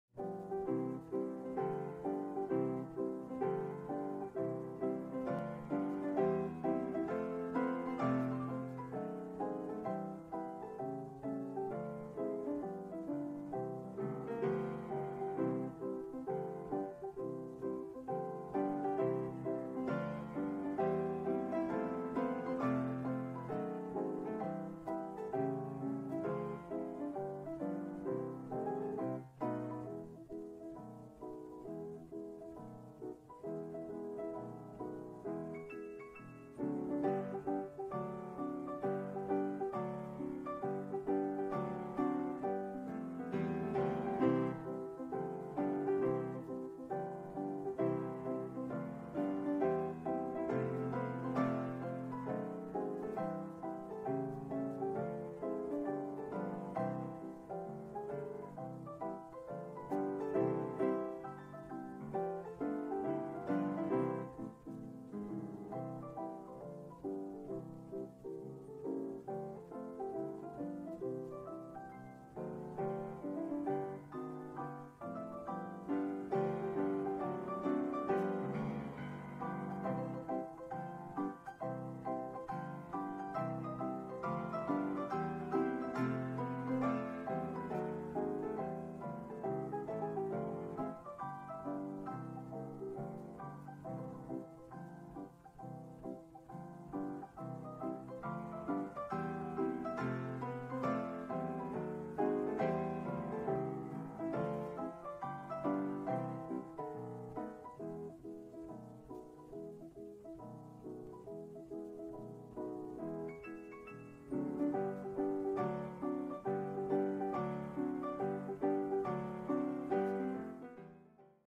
10 Things You Should Know About Anne Shirley
Here are 10 things you should know about Anne Shirley, born on April 17, 1918. She made more than 65 movies before retiring at age 26.